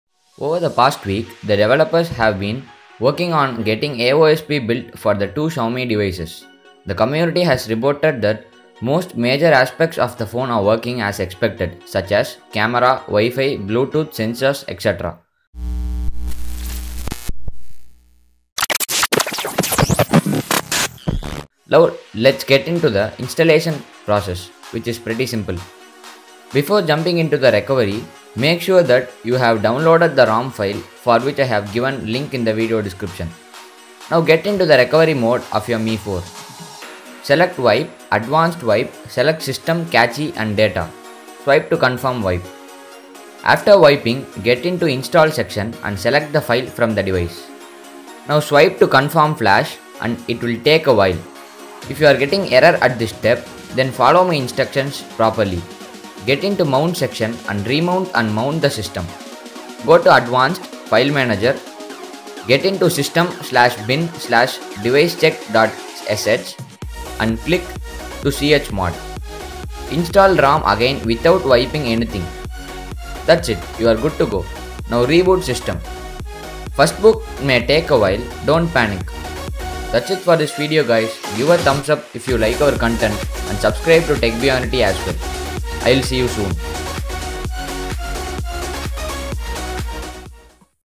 How to install Android 8.0 Oreo in xiomi mi 4 and mi 3 [cancro][Aosp][Twrp] !!
hey guys in this video i'll show how to install android oreo 8.0.0 in xiomi mi4 and oreo 8.0.0 in xiomi mi 3 [cancro] !! oreo mi 4 , oreo mi 3